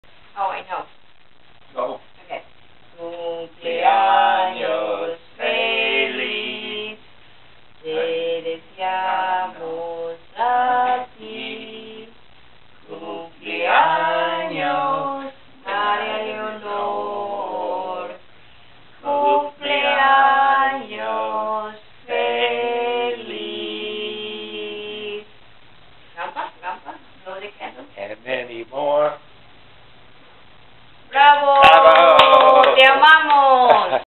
Happy B- day Maria Leonor !!! Te amamos. Dios te bendiga siempre
Maria Leonor esta cumpliendo 3 anitos hoy 21 de Junio !